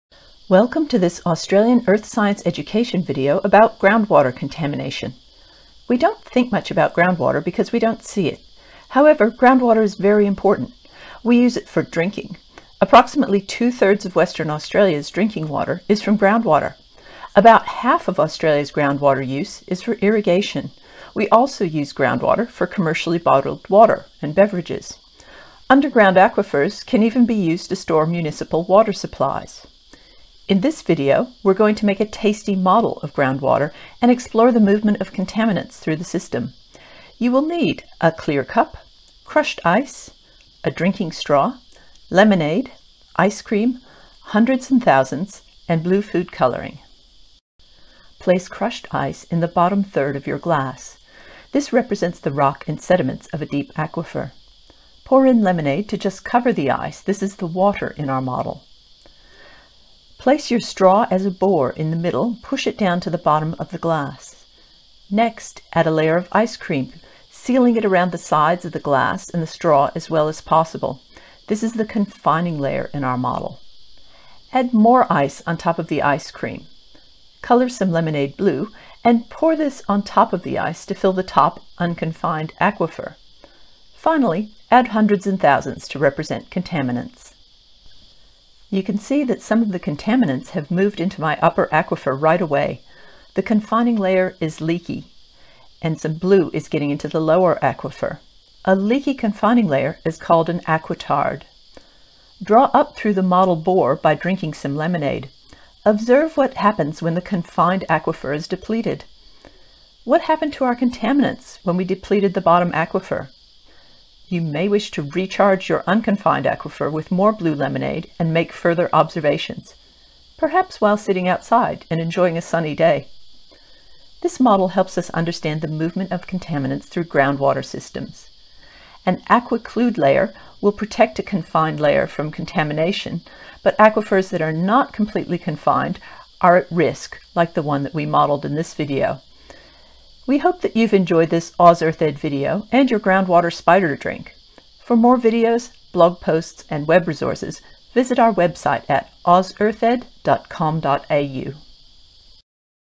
Groundwater Contamination
How can groundwater be contaminated? What is the difference between a confined and unconfined aquifer?
Thank you to the Ribbons of Blue program for the fantastic activity.